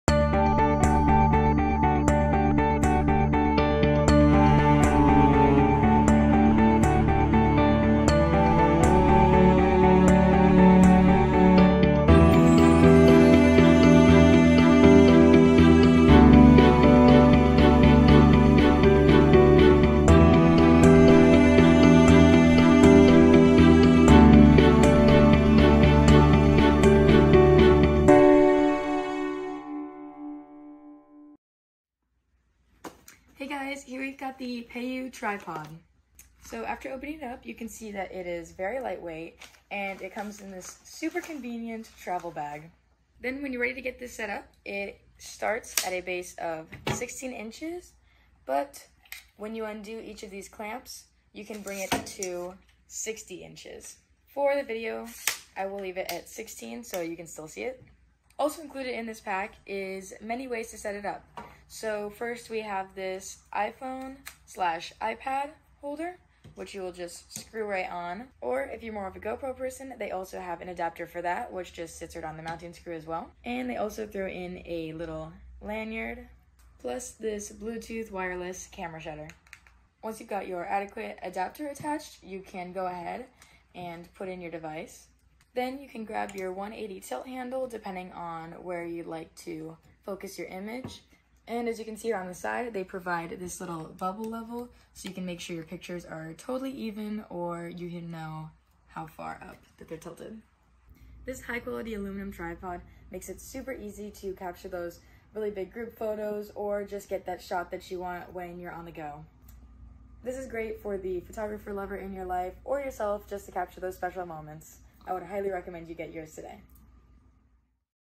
Tablet Tripod Stand Unboxing
Tablet Tripod Stand, 60" Extendable Aluminum Camera Tripod with Wireless Remote and Phone Holder, Compatible with Tablet/iPad Pro 12.9 inch/Webcam/Video Camera

Check out hot deals:
Amazon Store